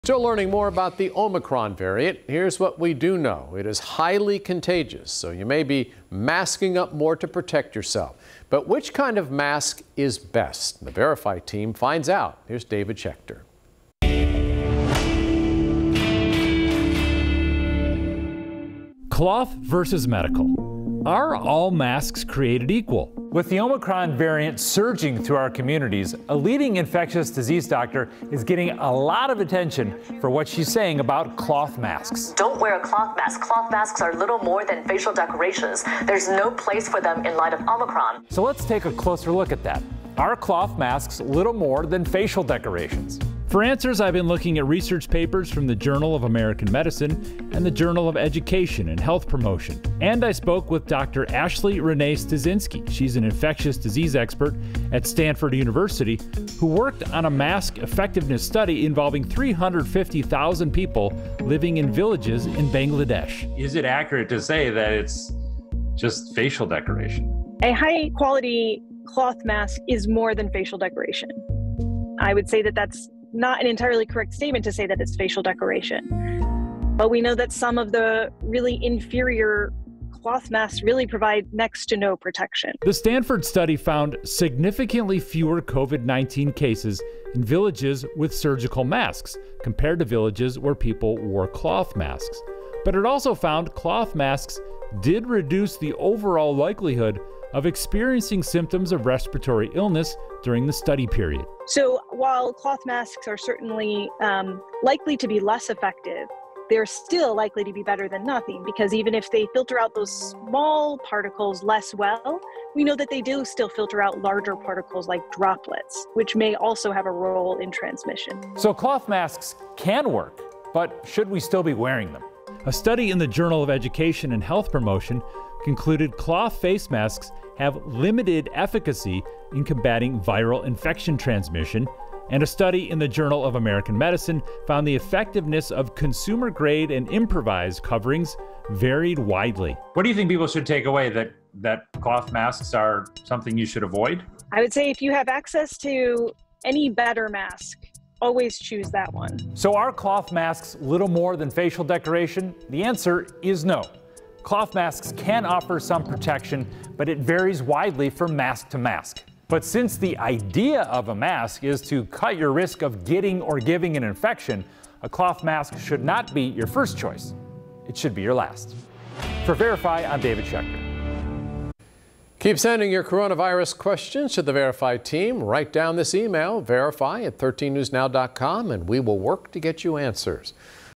VERIFY: Are cloth masks 'useless' against the omicron variant?
We're still learning more about the omicron variant of COVID-19. Here's what we do know: it's highly contagious, so you may be masking up more to protect yourself. But which kind of mask is best? The Verify team finds out. Here's David Schecter.